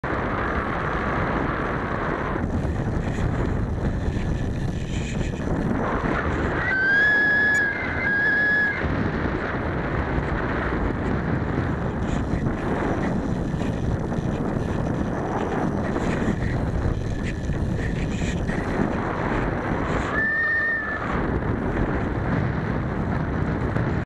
Red Tail Hawk harasses Allegro Lite
Red Tail Hawk lightly harasses Allegro Lite, then leaves without incident as Allegro yields airspace. Shot with 808 #16 cam with D lens.